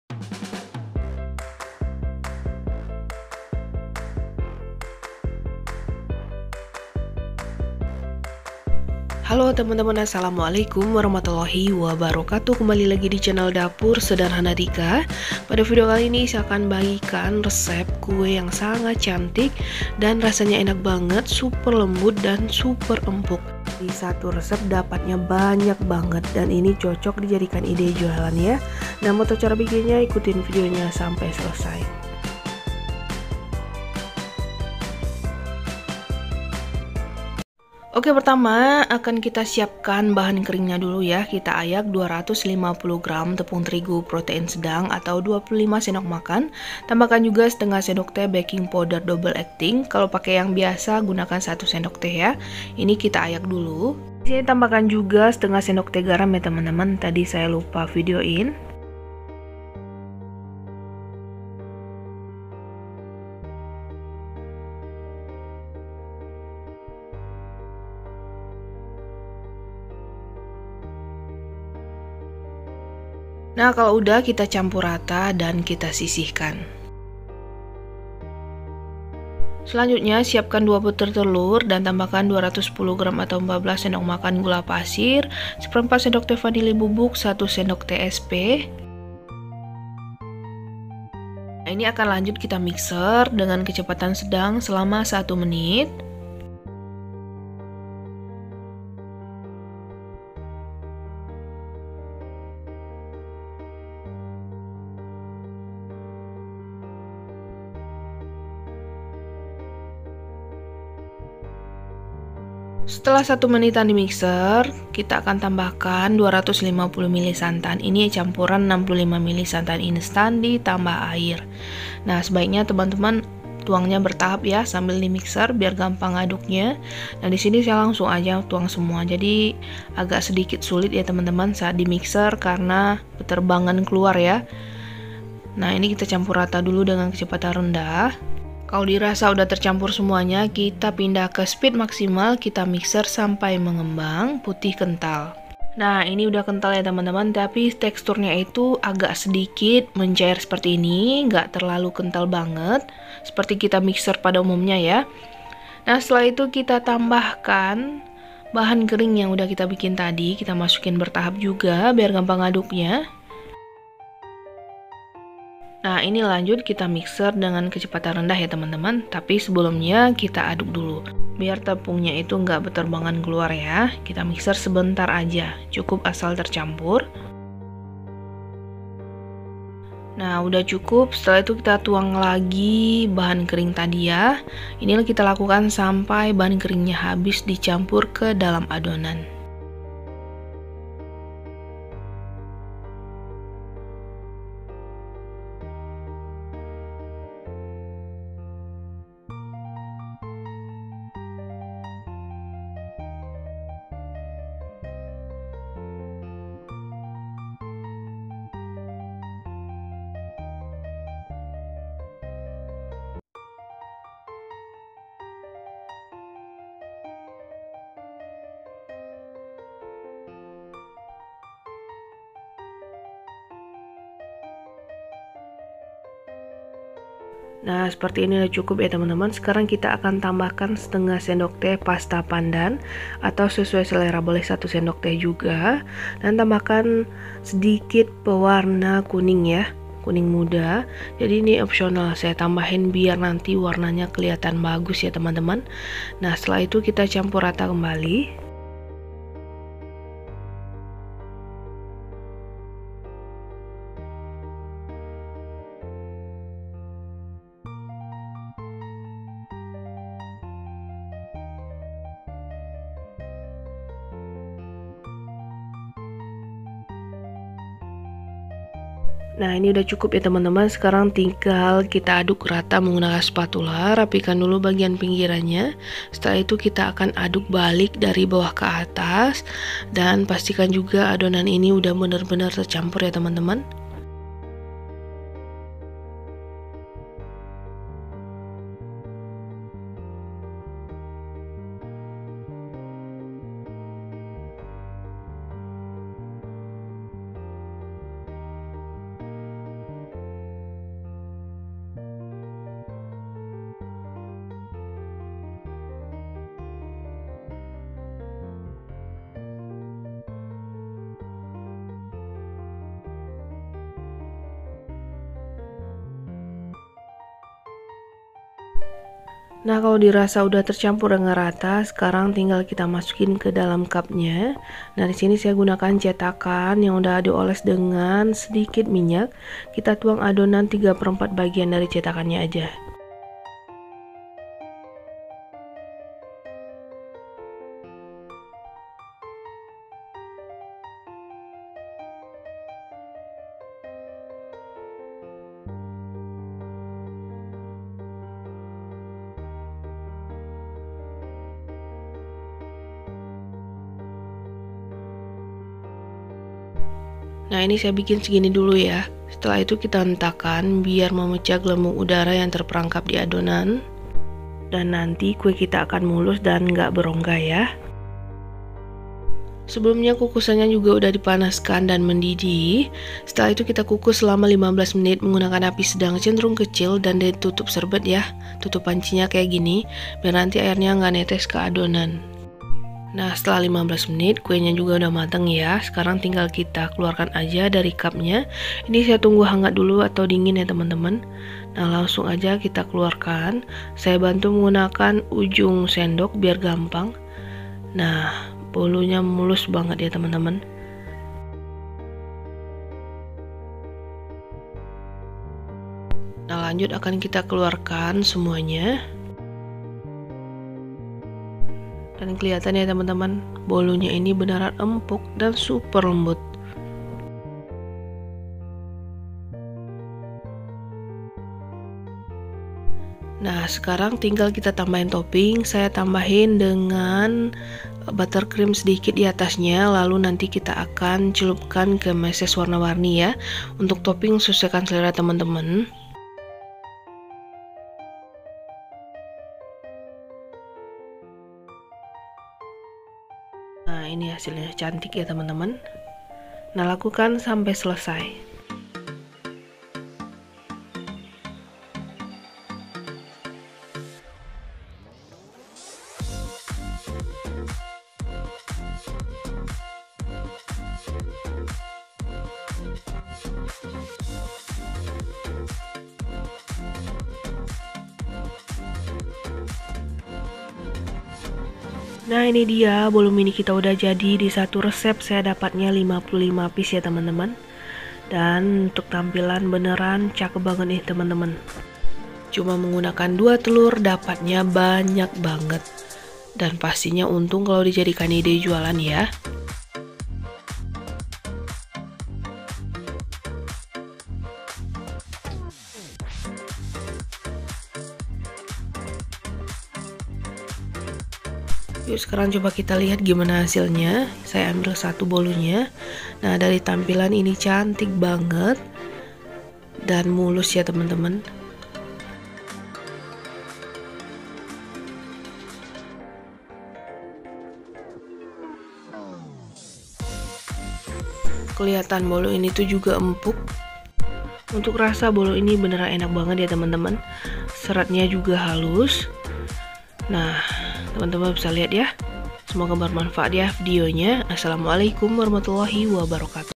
CUMA 2 TELUR JADI 55 pcs KUE‼️COCOK UNTUK TAMBAHAN IDE JUALAN KAMU | MODAL KECIL UNTUNG BERLAPIS
Bahan :

🌿 250 gr / 25 sdm tepung terigu pro sedang : 4.500
🌿 1/2 sdt baking powder double ackting / 1 sdt b.powder biasa : 500
🌿 1/2 sdt garam : 250
🌿 2 butir telur : 3.800
🌿 210 gr / 14 sdm gula pasir : 2.940
🌿 1 sdt sp : 1.000
🌿 1/4 sdt vanili bubuk : 250
🌿 250 ml santan ( 65 ml santan instan + air ) : 3.000
🌿 1/2 sdt pasta pandan / pewarna hijau
    + sedikit pewarna kuning ( opsional ) : 500
🌿 Buttercream : 1.500
🌿 meses warna warni : 3.000

Total Modal : 21.240
Total Kue : 55 pcs

Kalo mau bahannya lebih ekonomis,pake tepung yg kiloan aja ya teman teman,, buttercream juga bisa diganti margarin oles tipis aja..

Selamat mencoba, semoga bermanfaat ya..